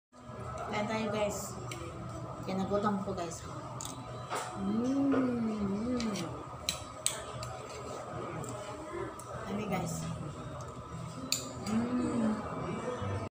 Paspas kaon day bisag init tulon.
Ang babayeng gigutum nagsenerok